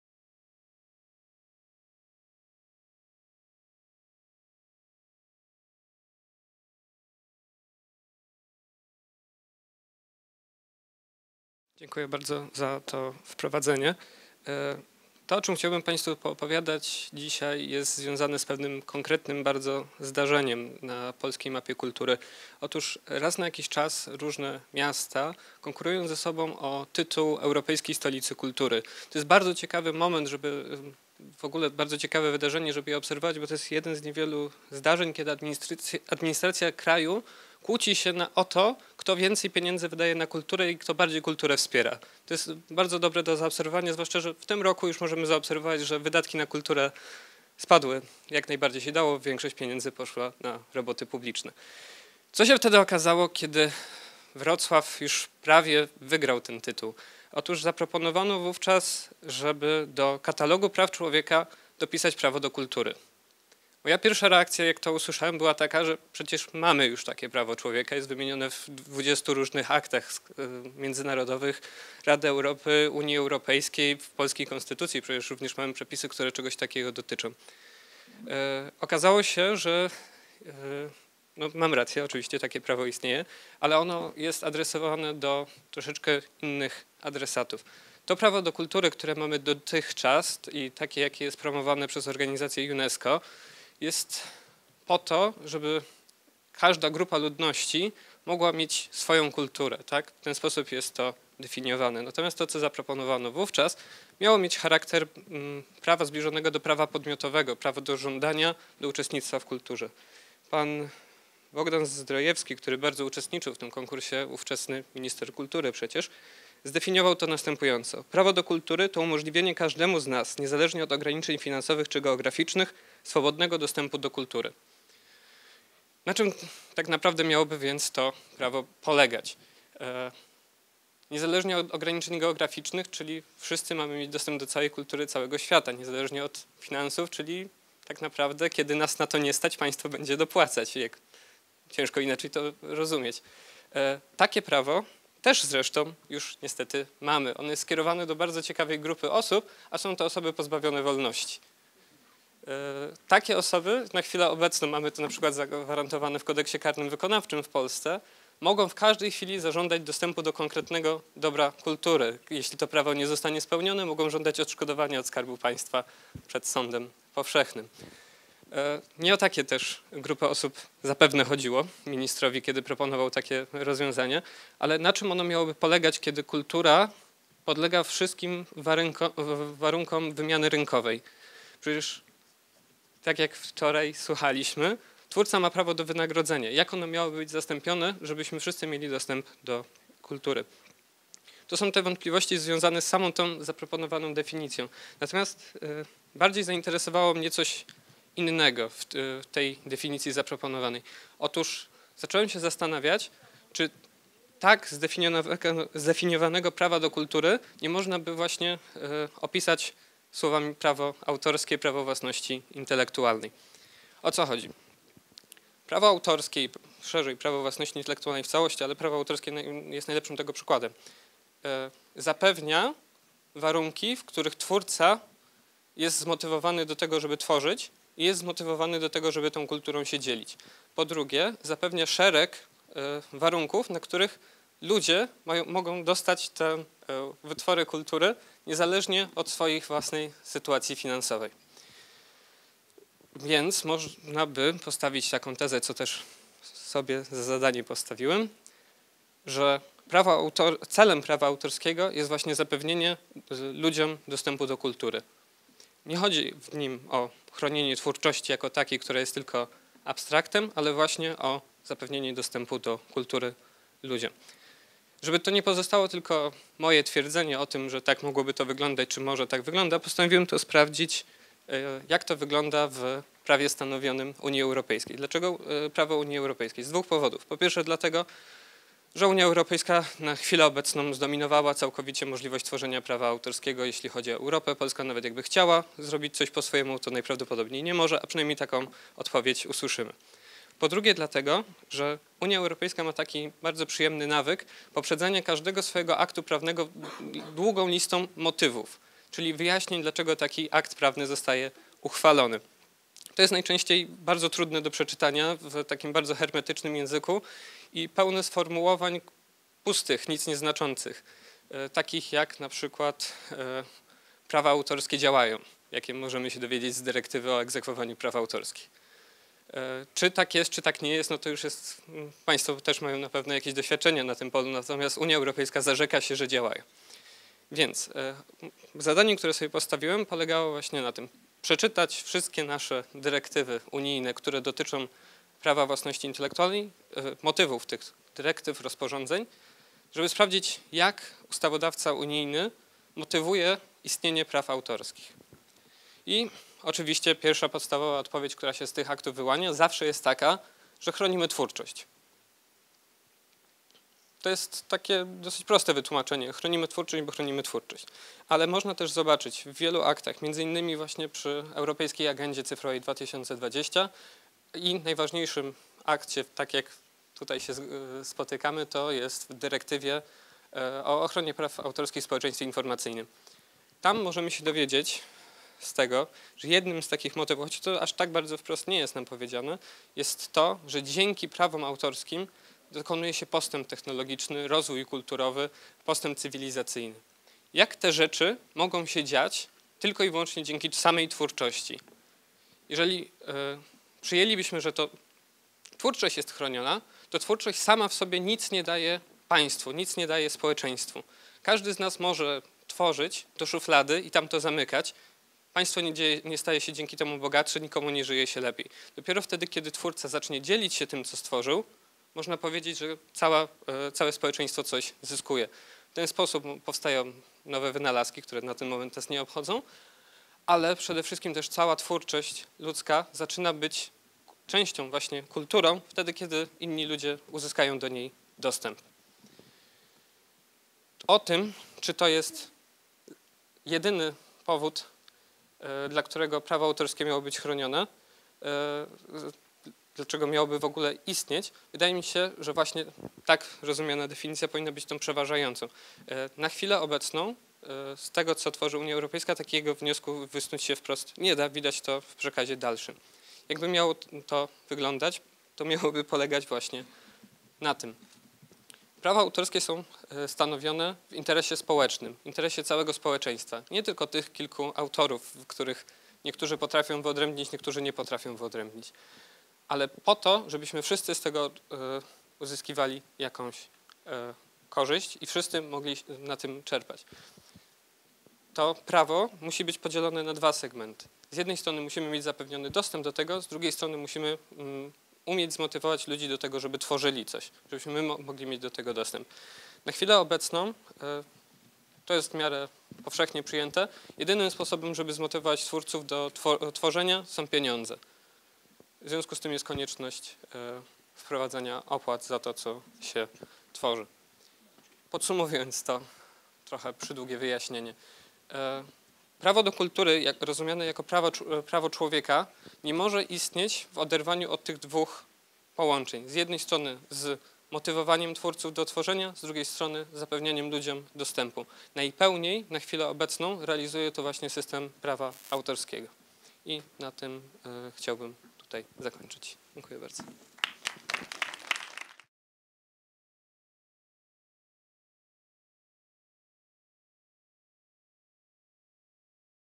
Maciej Giermak - CopyCamp 2014
Wystąpienie Macieja Giermaka "Prawo do kultury w prawie Unii Europejskiej" na konferencji CopyCamp, która miała miejsce 6-7 listopada 2014 w Warszawie.

Maciej Giermak – absolwent Wydziału Prawa i Administracji Uniwersytetu Opolskiego, aplikant radcowski przy Okręgowej Izbie Radców Prawnych w Opolu.

Presentation of Maciej Giermak "Right to culture in the EU law" at the CopyCamp conference which took place on the 6th and 7th of November 2014 in Warsaw.

Maciej Giermak – graduate of the Faculty of Law and Administration at the University of Opole, trainee solicitor at the District Chamber of Legal Advisers in Opole.